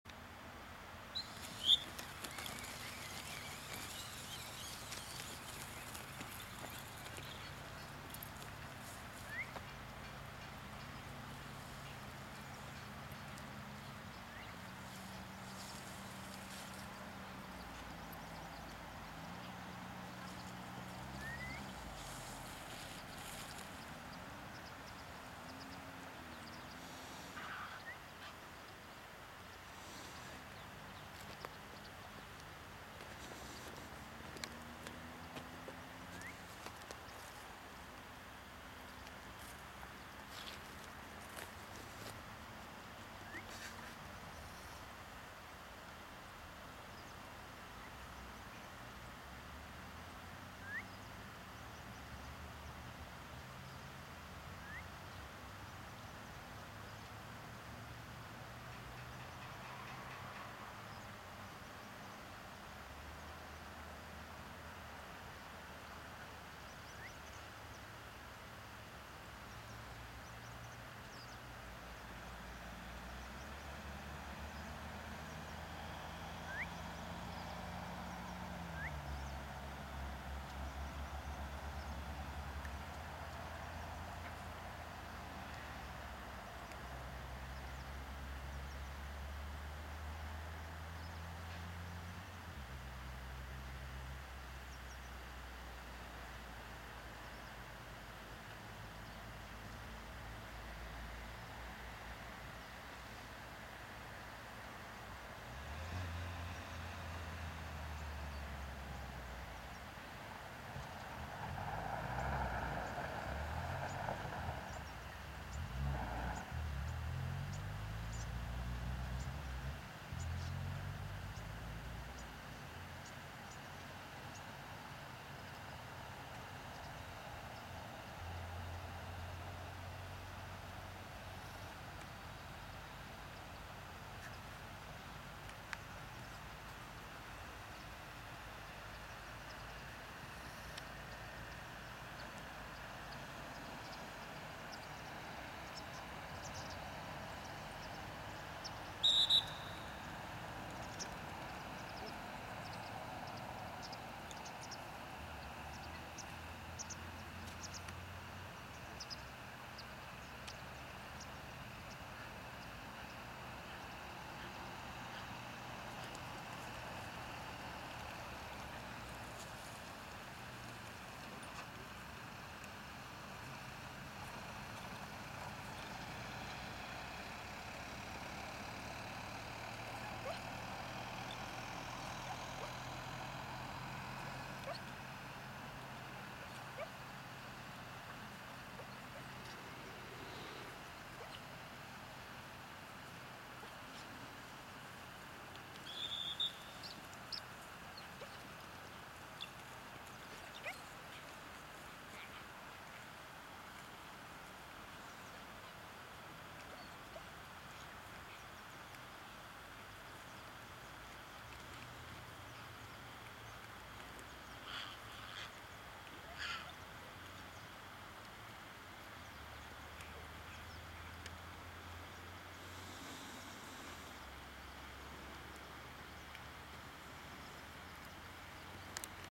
Elhew Jake
無風状態でコンディションがあまり良くなかった。
ジェークのポイント姿勢はベンチワーク訓練で簡単に矯正する事はできますが？今はその時期ではありません（自然鳥を経験すれば何の問題もありません。それがエルフューポインターなのです）？

後ろで繋がれているミッジがバッキングに入りかけていますが、興奮しすぎて尻尾がなかなか止まりません（クラッキング状態）。この現象は来日以来段々酷くなっていますが、
これ以上悪くなる事は無いでしょう。
最大の原因は毎日チェックコードで行う運動が引き起こすストレスです。